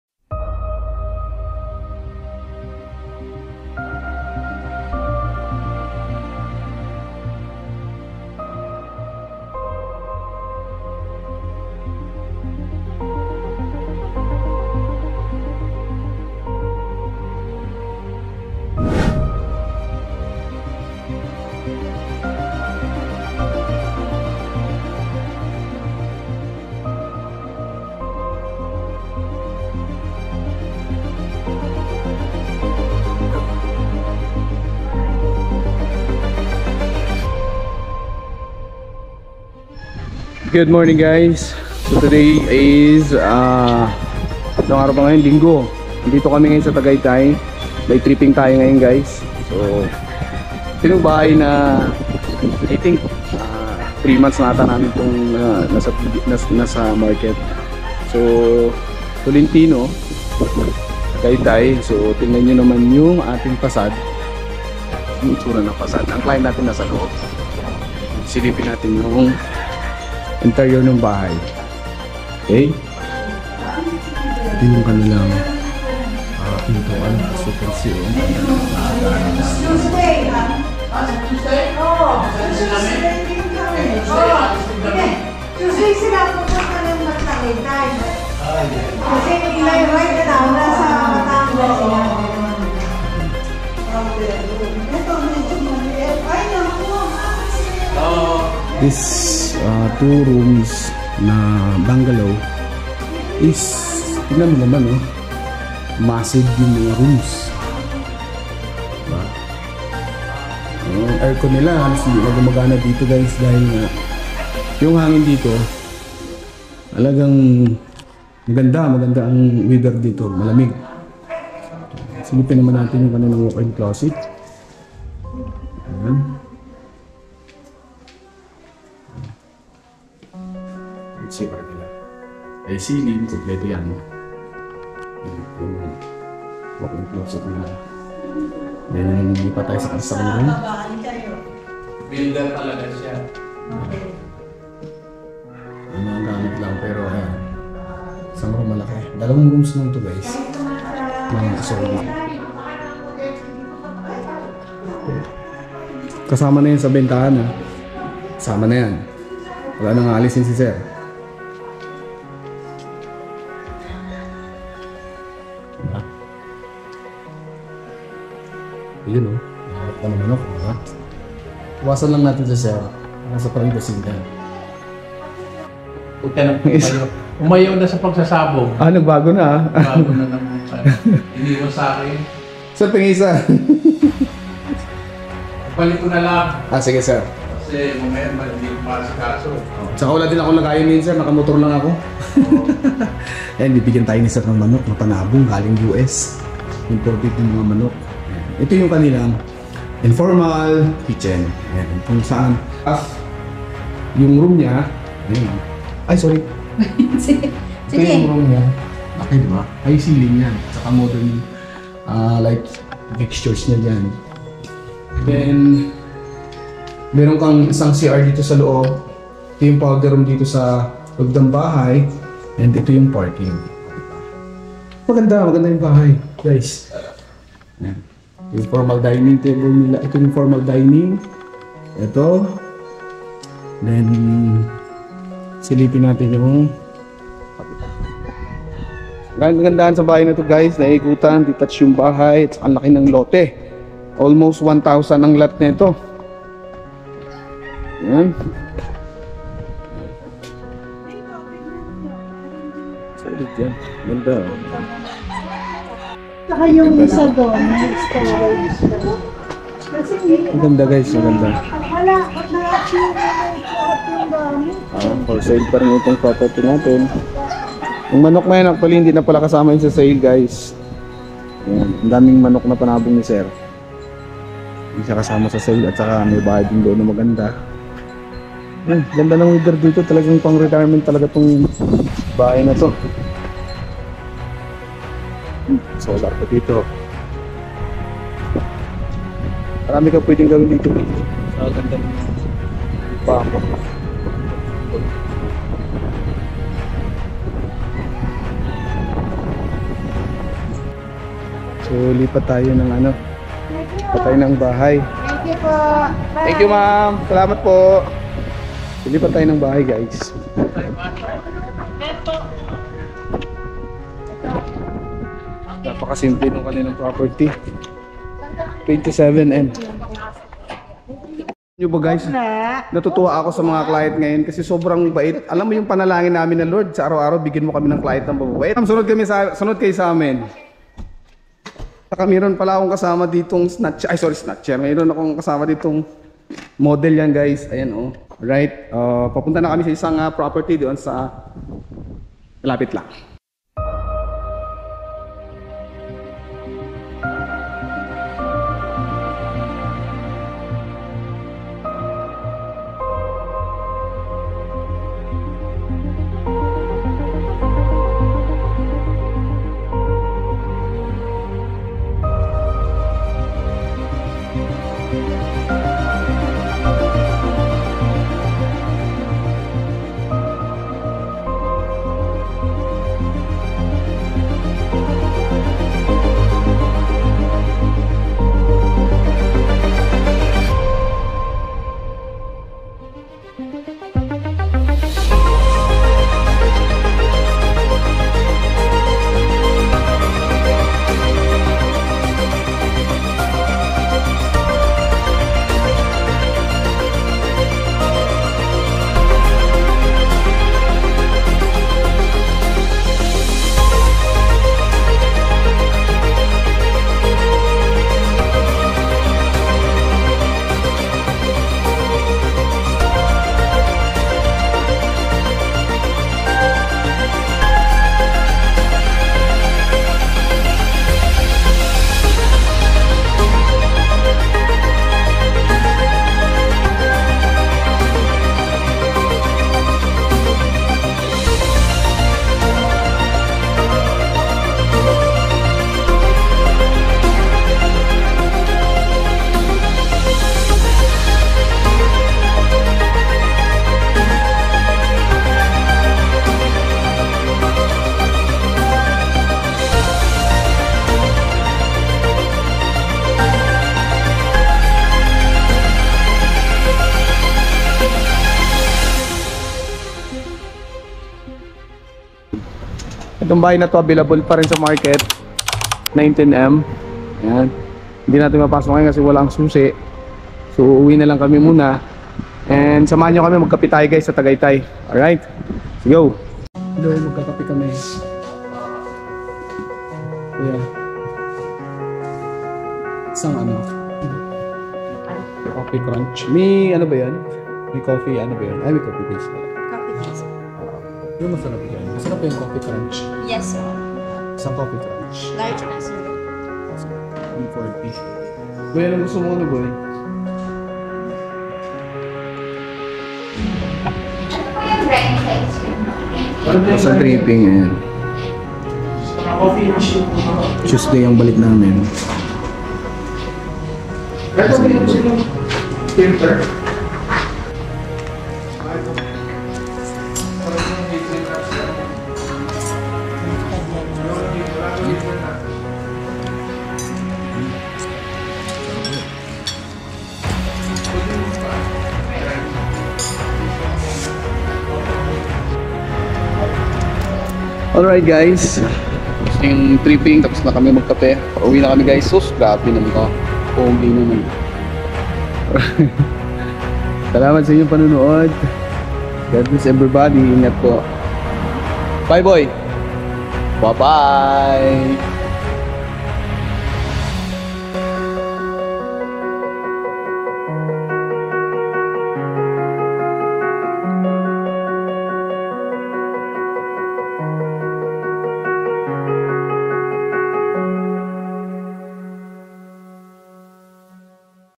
TRIPPING DAY | BRAND NEW HOUSE | TAGAYTAY CITY
HELLO GUYS SAMAHAN NYO KAMI SA AMING BAGONG TRIPPING DITO SA TAGAYTAY CITY, ITO YUNG MGA LINK NANG BAHAY NA AMING PINUNTAHAN SA TAGAYATAY, KUNG NAIS NYO MAPANUOD ANG FULL VIDEO ITO ANG LINK,
MARAMING SALAMAT SA INYONG SUPORTA